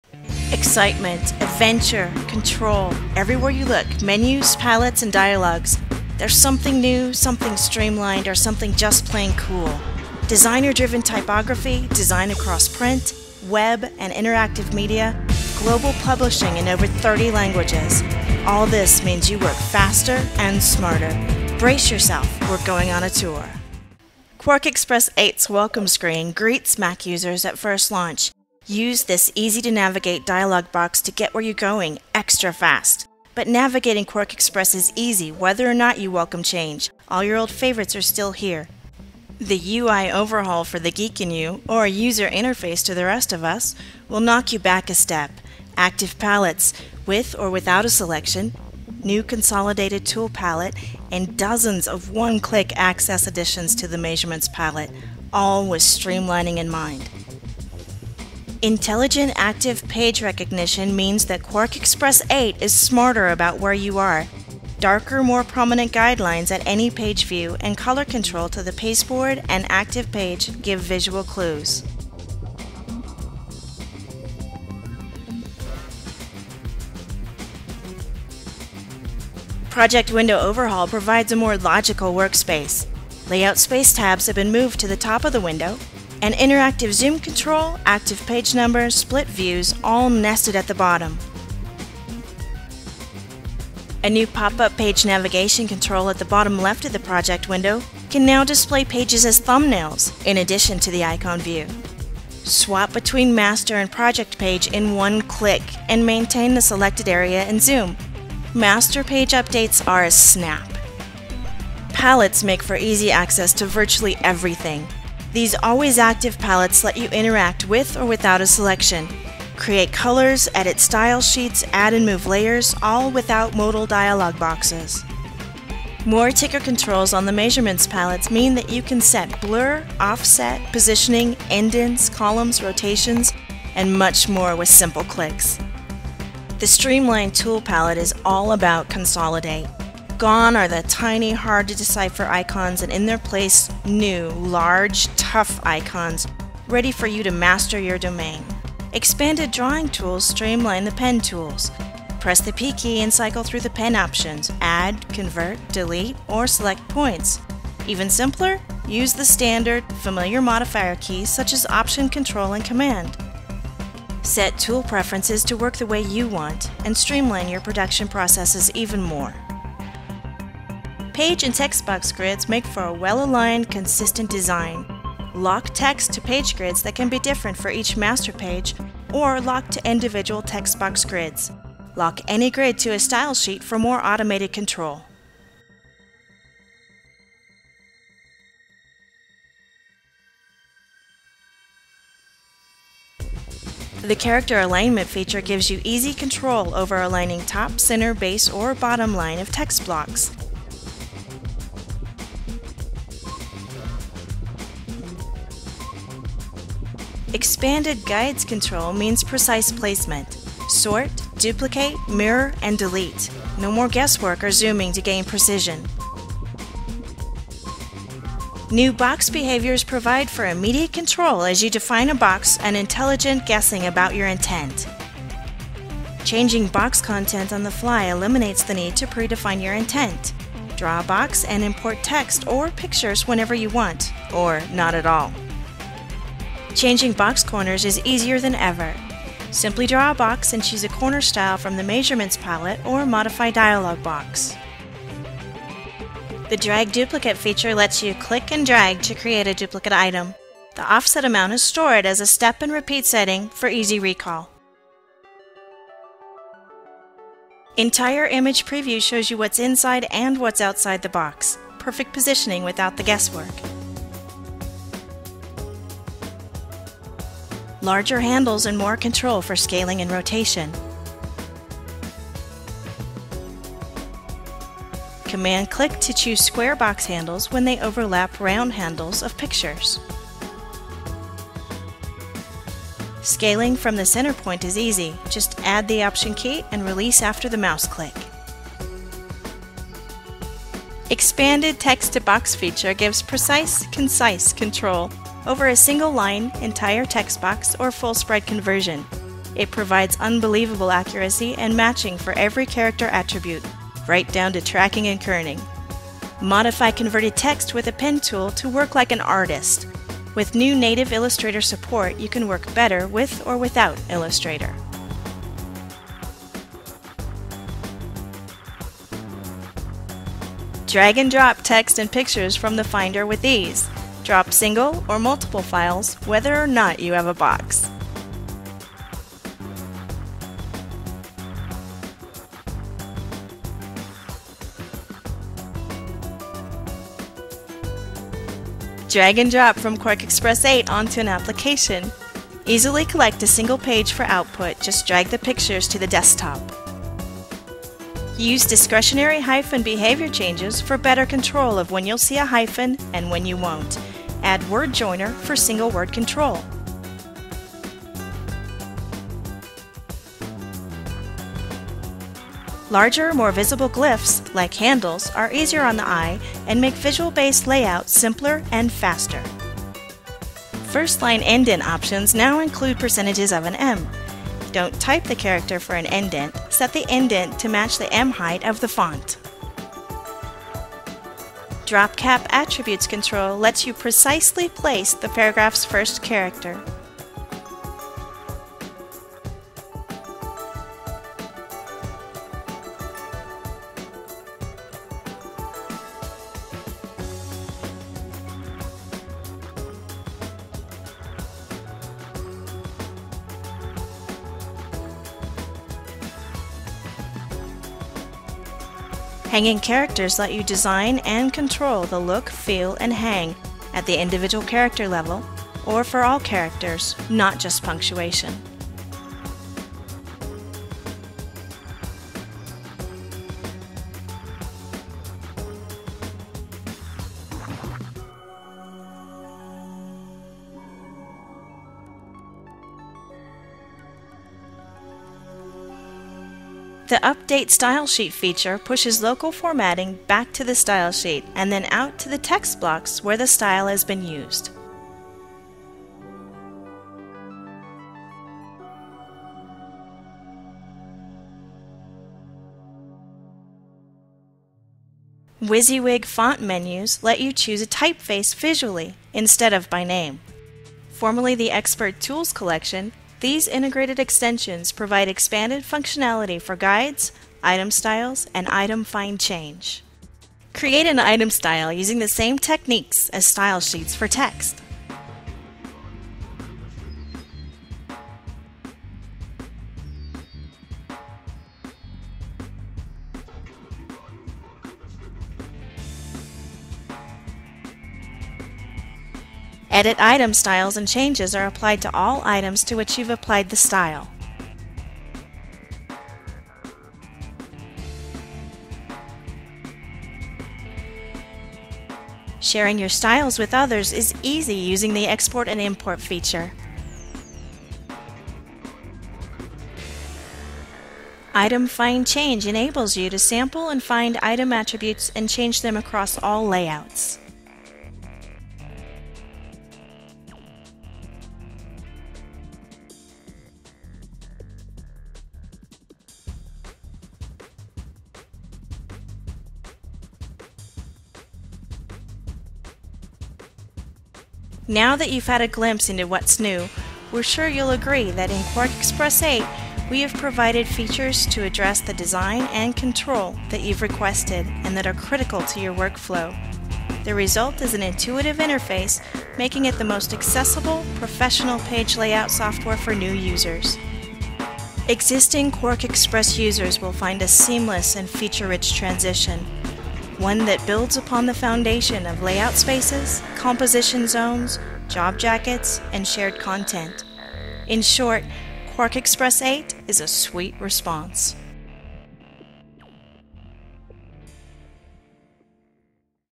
Was ist neu in QuarkXPress 8?
In diesem Video sehen Sie die neuen Funktionen von QuarkXPress 8 in Aktion. Experimentieren Sie mit der Benutzeroberfläche, um sich mit Gruppen für hängende Zeichen, Seitenrastern, der Steuerung per Tastatur, Objektstilen und vielem mehr vertraut zu machen.